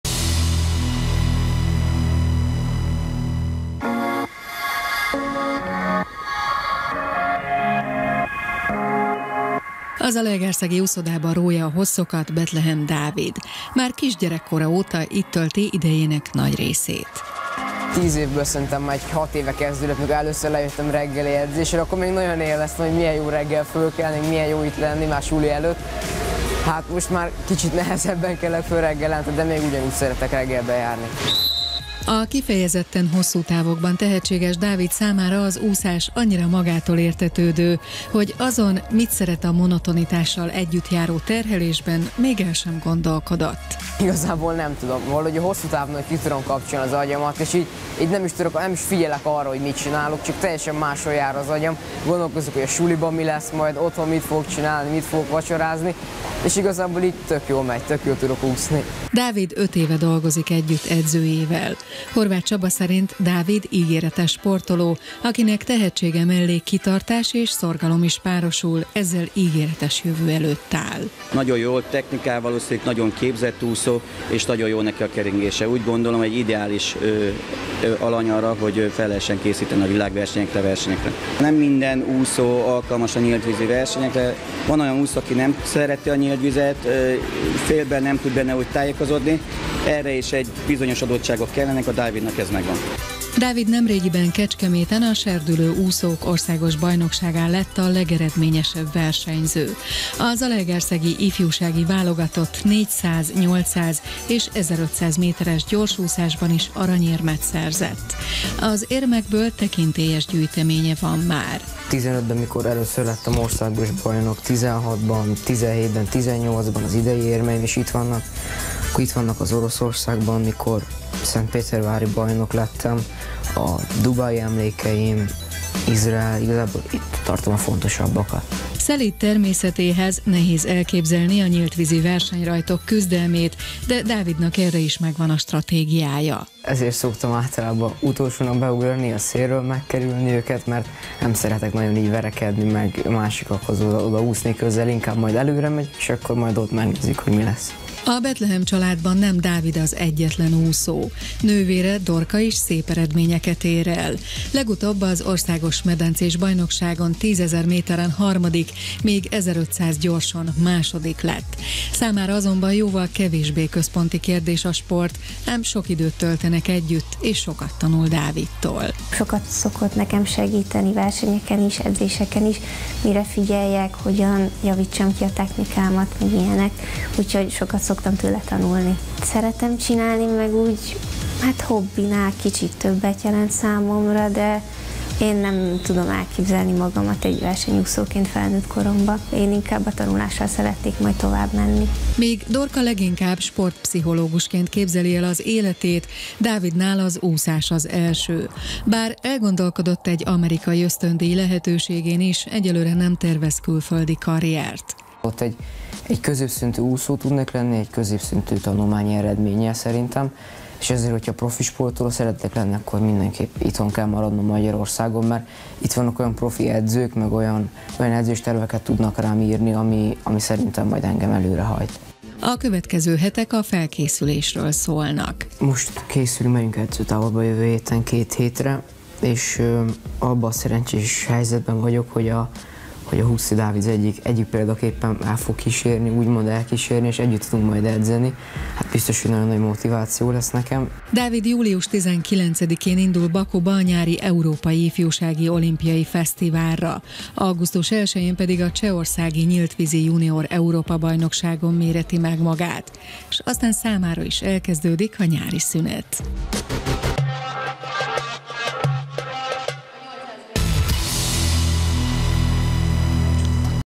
Betlehem Dávid - bemutatjuk a jövő úszóbajnokát!
A zalaegerszegi kamasz, Betlehem Dávidminden túlzás nélkül a jövő nagy úszóbajnoka is lehet! A hosszabb távokban tehetséges fiú halmozza a győzelmeket, 2018-ban Jeruzsálemben, a Nyíltvízi Junior Világbajnokságon lett világbajnok harmadik helyezett. Dávid nemcsak edzésére, hanem az otthonába is beengedte a Petőfi TV stábját, ismerjétek meg őt!